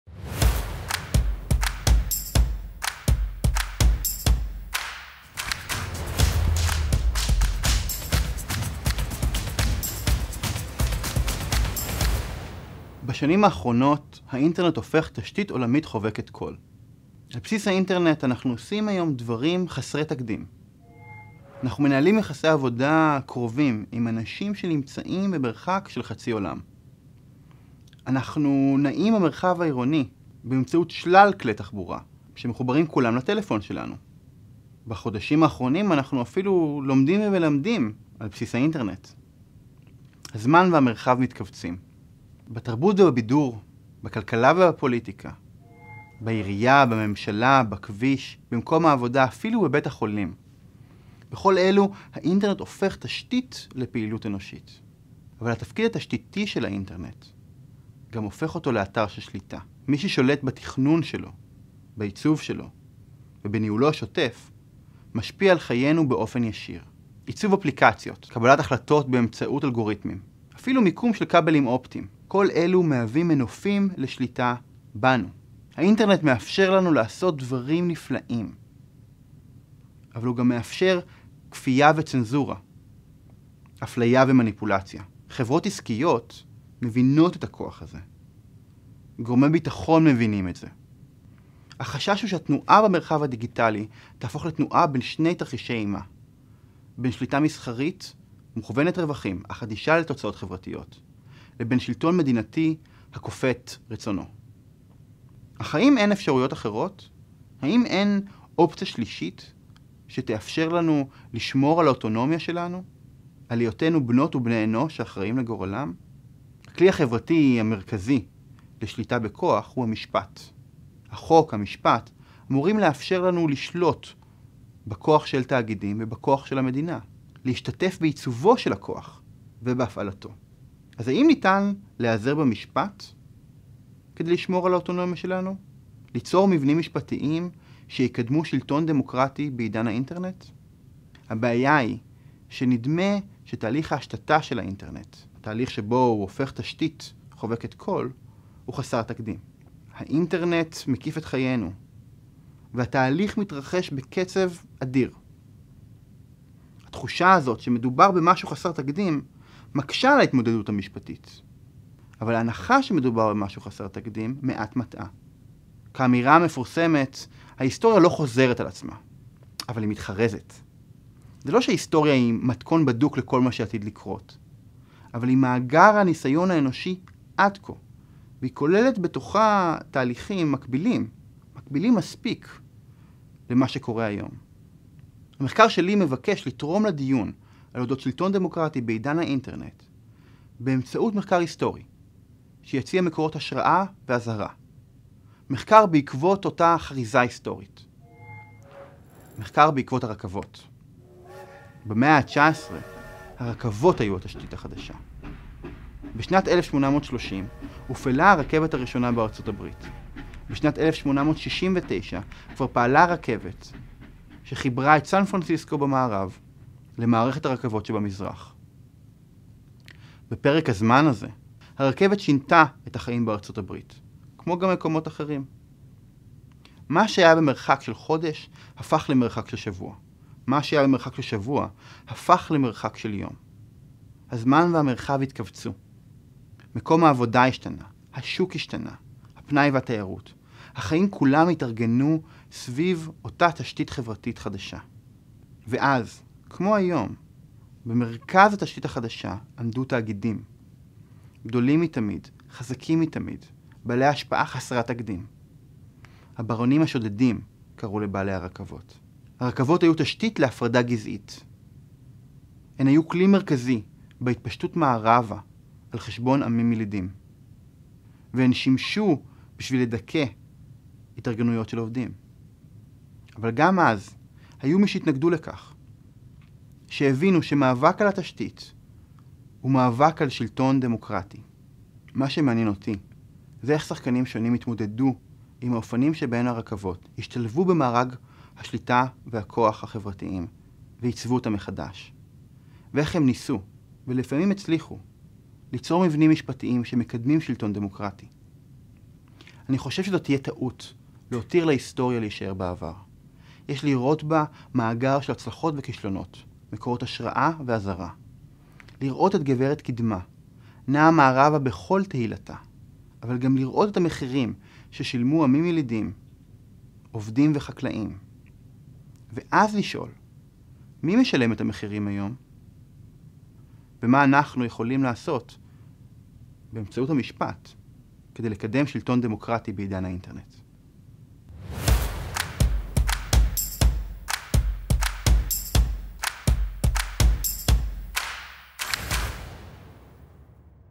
אלון חספר Alon Jasper | האינטרנט ודמוקרטיה: לקחים מעידן הרכבות
MindTalks
פרויקט של מרכז מיינדוקייט למחקר וחדשנות במדעי הלמידה, אוניברסיטת תל אביב
A Project of Minducate, Science of Learning Research and Innovation Center, Tel Aviv University, Israel.